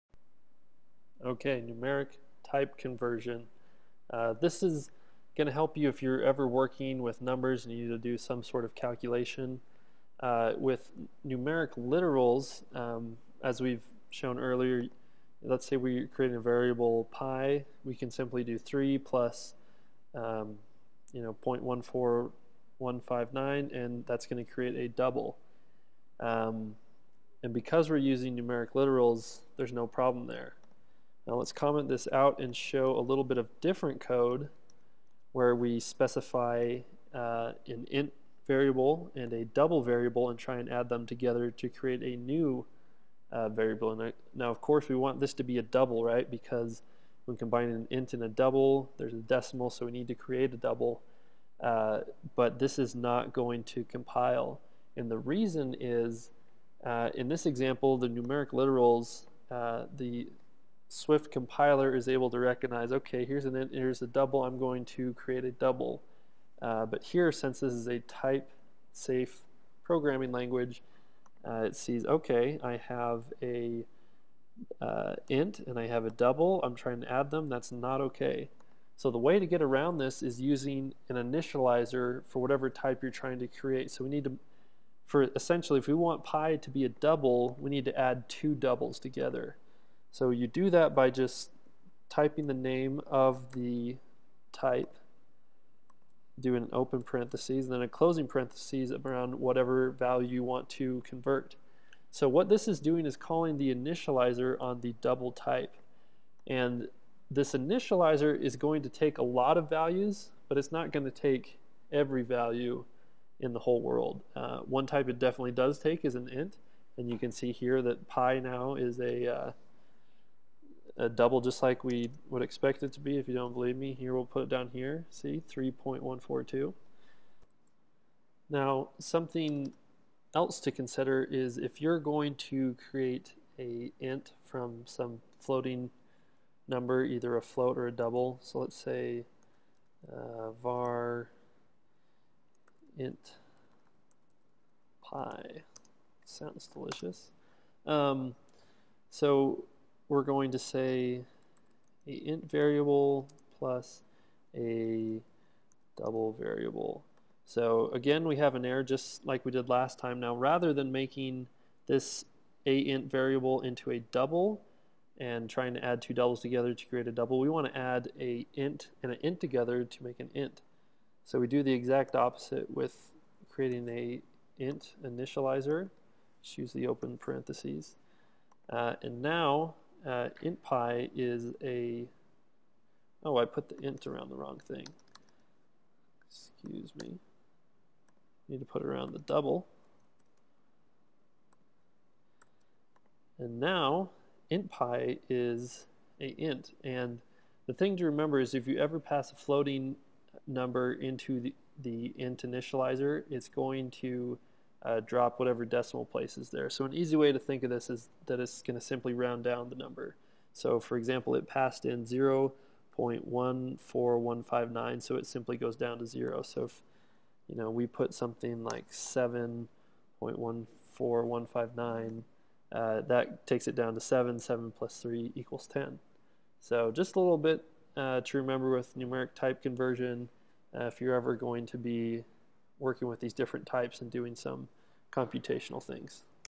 15. Numeric Type Conversion | Swift Course on Udemy
In this course by Rick Walter on Udemy, you will learn Apple's Swift programming language by examples.